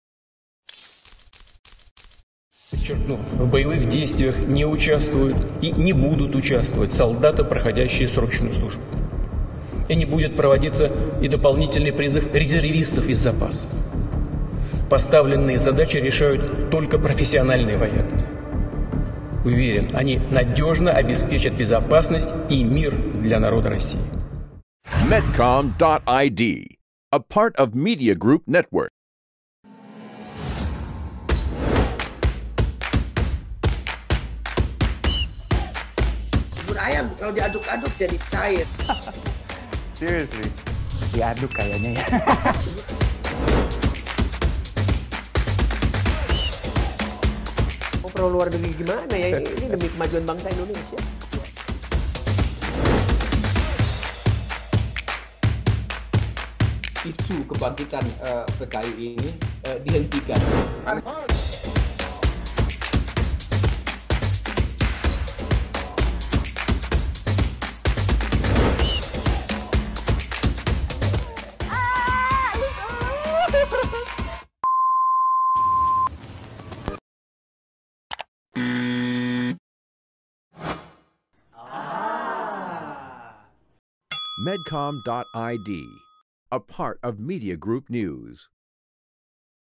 Putin Tegaskan Tak akan Kerahkan Tentara Wajib Militer
Dalam sebuah video yang dirilis pada Selasa (8/3) dini hari, Presiden Rusia Vladimir Putin mengatakan tidak akan mengerahkan tentara wajib militer dalam operasi tempur di Ukraina. Tujuan yang ingin dicapai di Ukraina hanya akan dilakukan oleh tentara profesional. APTN.